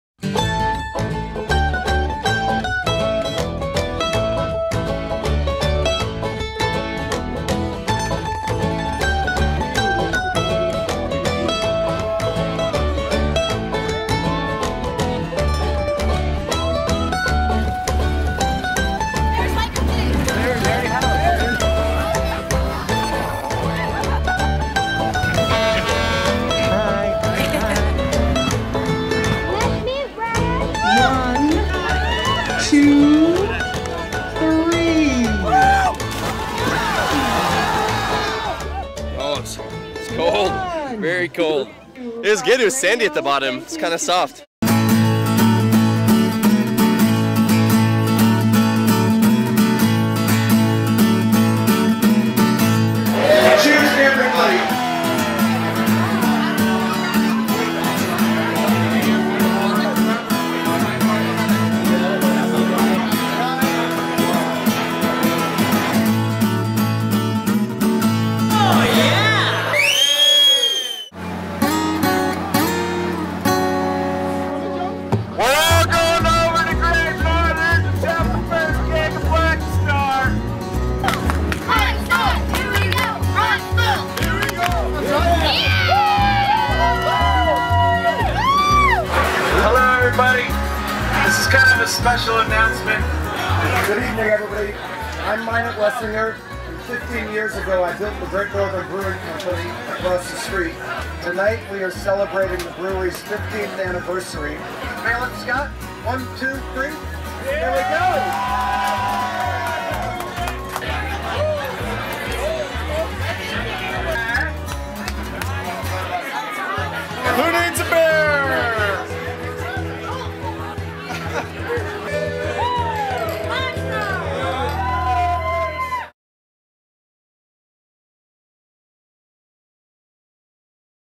Black Star Beer - 15 Year Anniversary Party
On February 6, 2010, friends of Black Star welcomed the double hopped golden lager back to Whitefish, Montana, where the journey began 15 years ago.